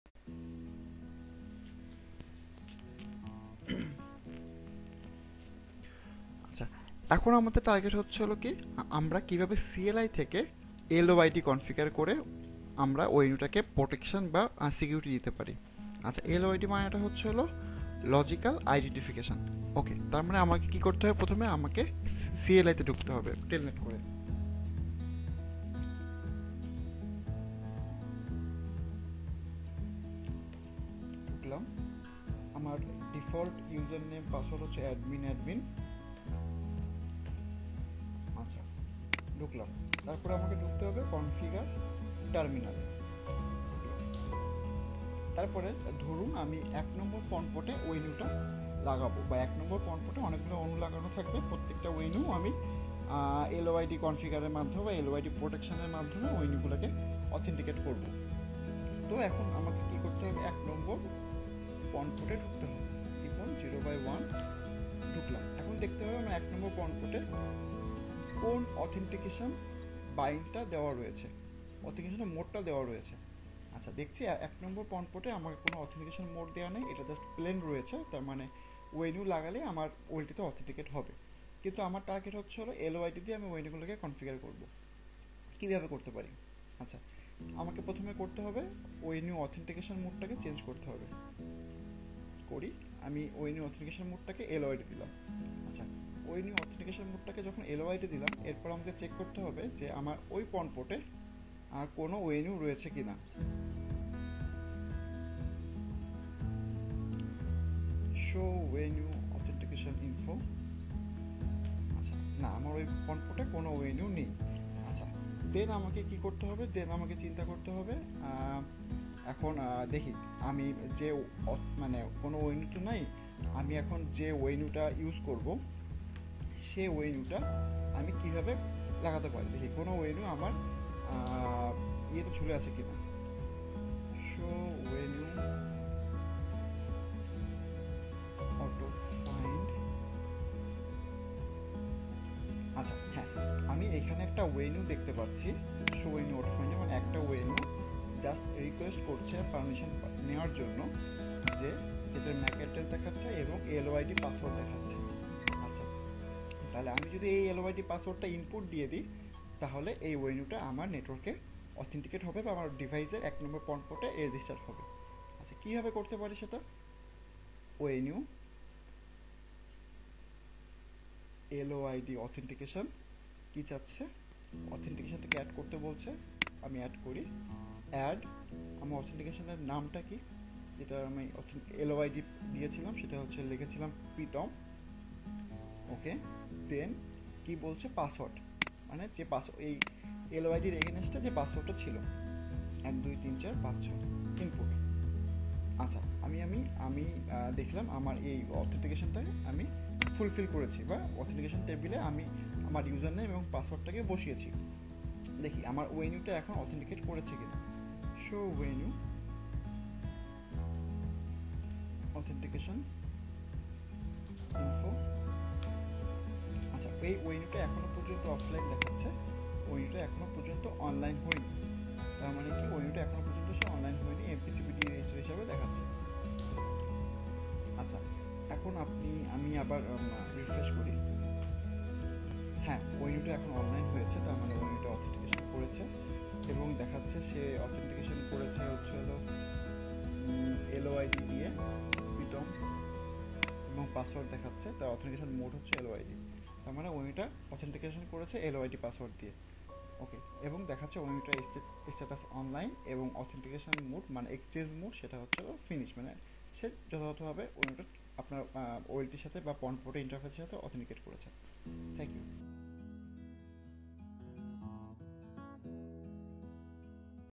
How to LOID (Logical Identification) Based ONU Authentication at CLI in VSOL EPON OLT (T-026)
ONU LOID-Based authentication
epon-olt(config-pon-0/1)# show onu auth-mode
epon-olt(config-pon-0/1)# onu-auth mode loid
epon-olt(config-pon-0/1)# show onu auto-find
epon-olt(config-pon-0/1)# onu loid-auth add epon 123456